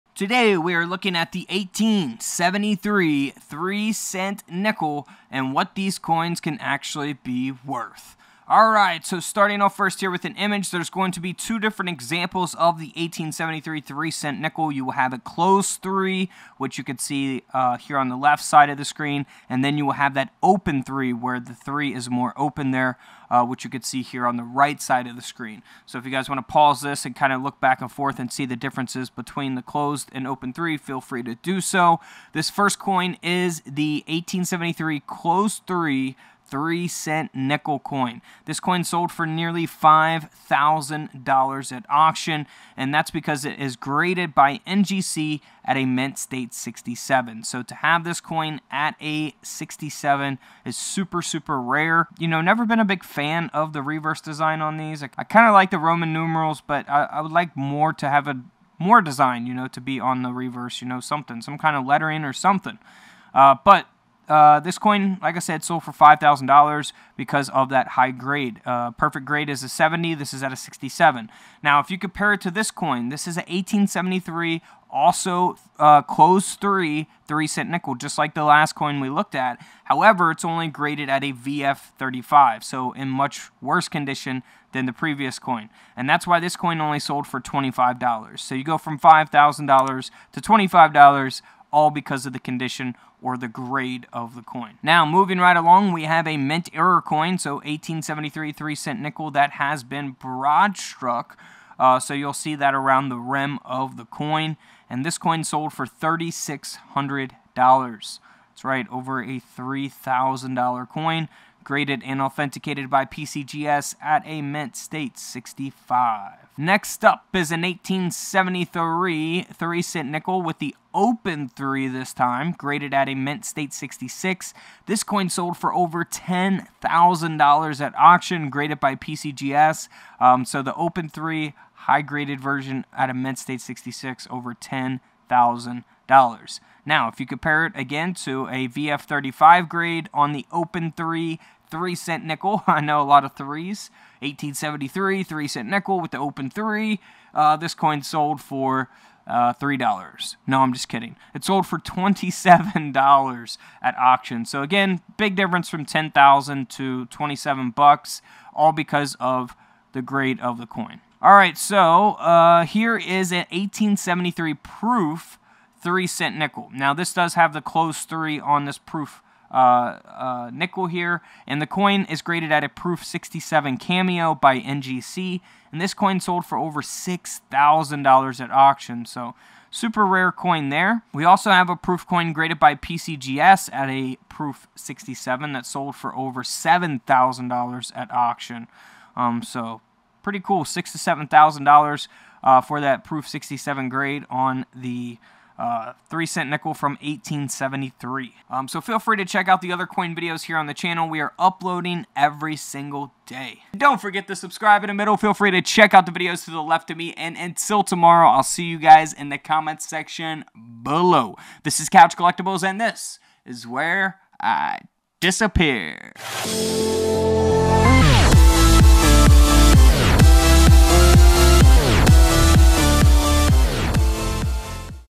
3 Cent Nickels Worth Thousands of Dollars!
3 Cent Nickels Worth Thousands of Dollars! These are old valuable coins that are worth money because of their mint errors and high coin grades.